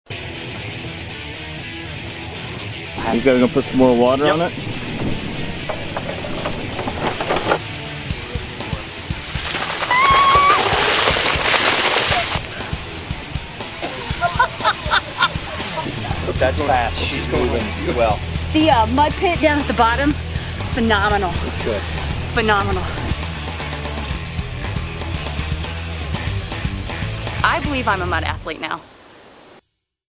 Mudathlon commercial :30 sec
Check out obstacle #17 at the upcoming Mudathlon Indy on June 26th